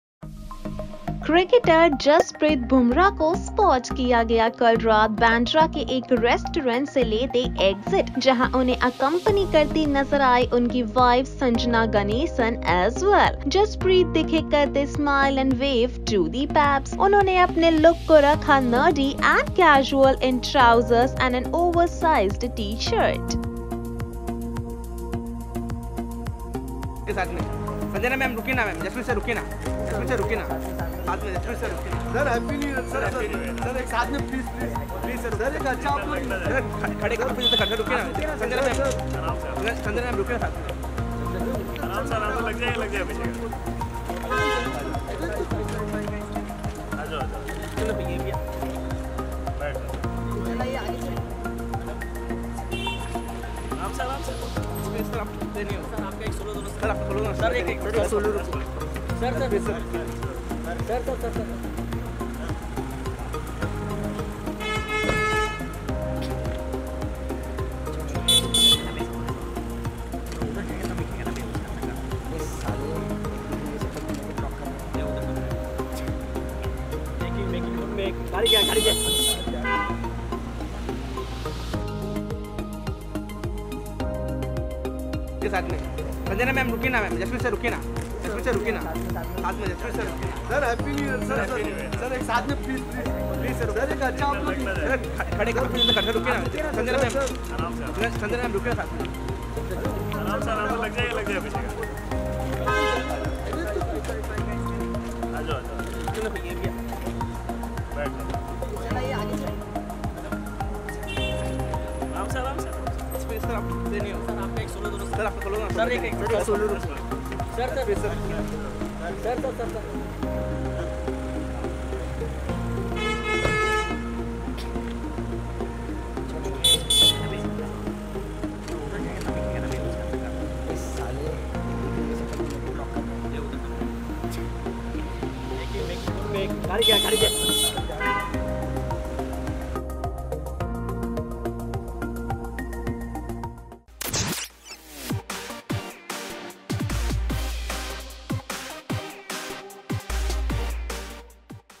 Jasprit Bumrah With Wife Sanjana Ganesan Spotted At Mizu Restaurant In Bandra
Copyrights@BoogleBollywood